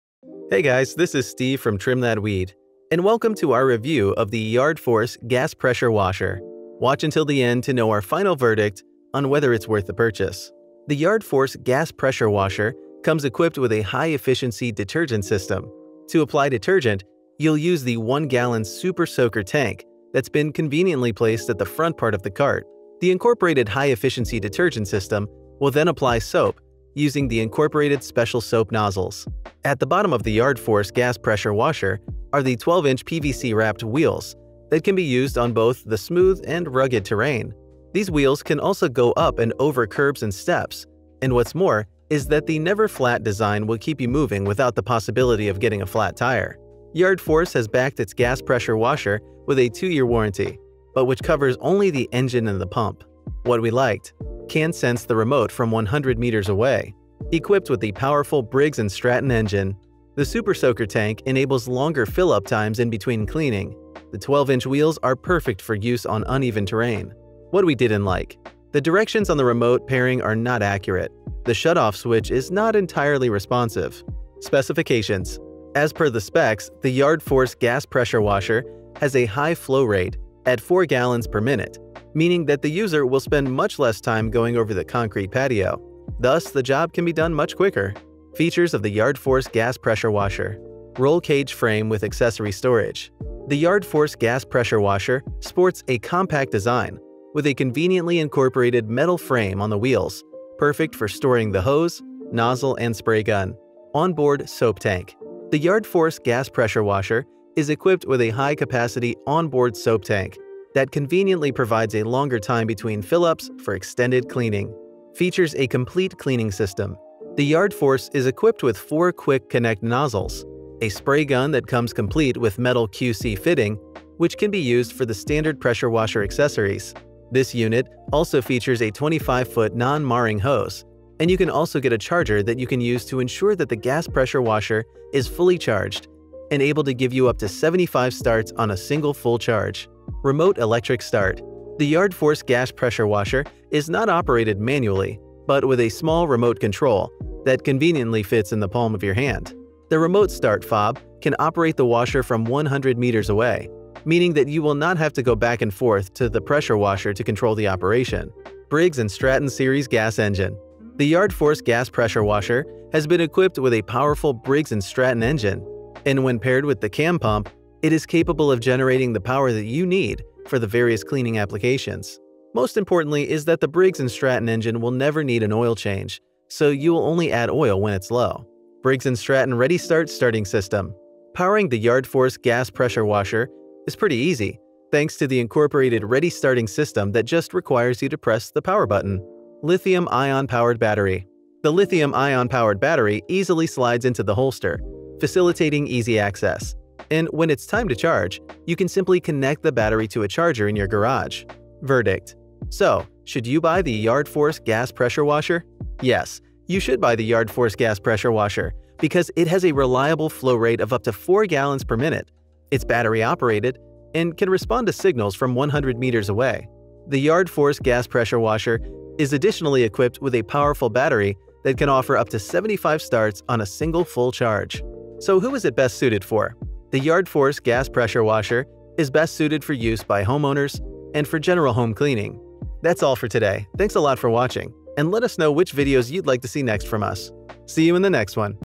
Yard Force Gas Pressure Washer (YF3100ES-R) - Best Way to Clean Your Concrete Patio
Yard Force Gas Pressure Washer (YF3100ES-R) - Best Way to Clean Your Concrete Patio. Welcome to our review of the Yard Force Gas Pressure Washer (YF3100ES-R).

The Yard Force Gas Pressure Washer comes equipped with a high efficiency detergent system. To apply detergent you will use the 1 –gallon super soak tank that has been conveniently placed at the front part of the cart. The incorporated high efficiency detergent system will then apply soap using the incorporated special soap nozzles.

At the bottom of the Yard force gas pressure washer are the 12” PVC wrapped wheels that can be used on both the smooth and rugged terrain. The wheels can also go up and over curbs and steps, what’s more is that the never flat design will keep you moving without the possibility of getting a flat tire. Yard force has backed its gas pressure washer with a 2 year warranty but which covers only the engine and the pump.